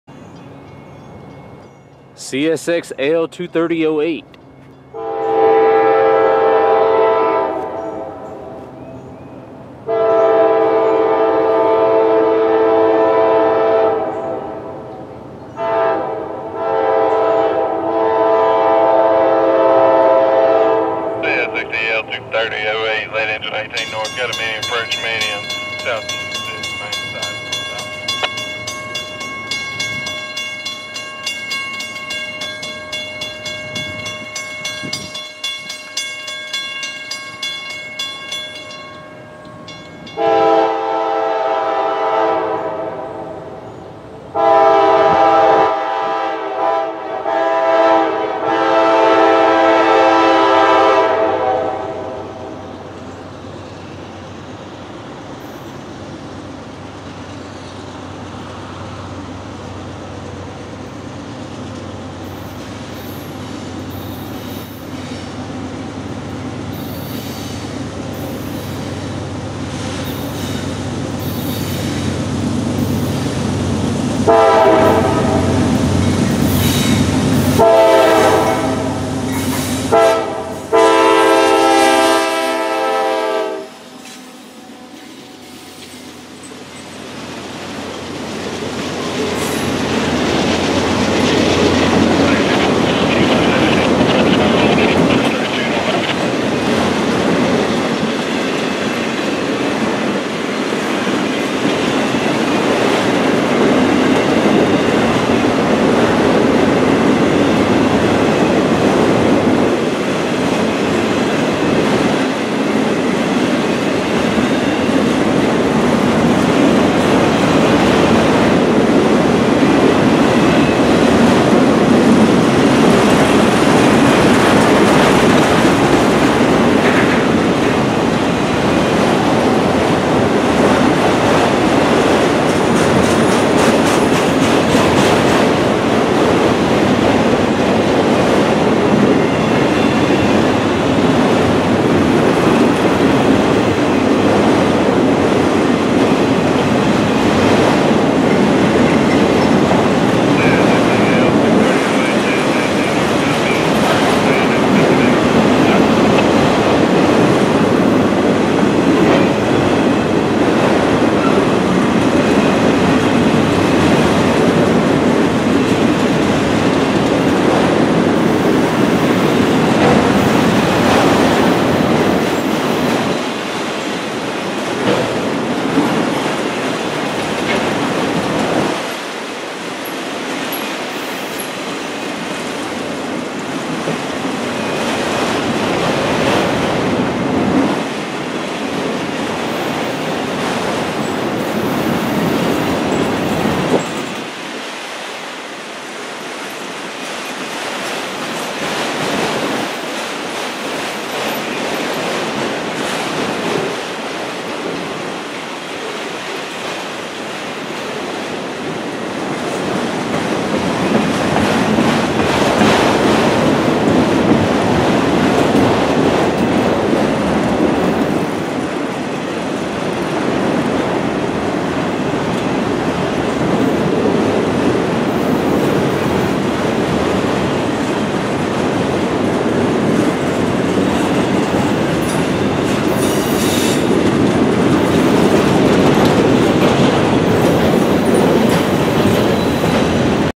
CSX L230-08 with a trailing NS SD70 at Mayors Park in Union City, GA 5/9/14
CSX L230 safely heads up the siding track and onto the Stonewall Connection track past Mayors Park with a CSX AC4400CW and a NS SD70.

CSX L230-08
12:33PM
CSX AC4400CW 18 K5LAR24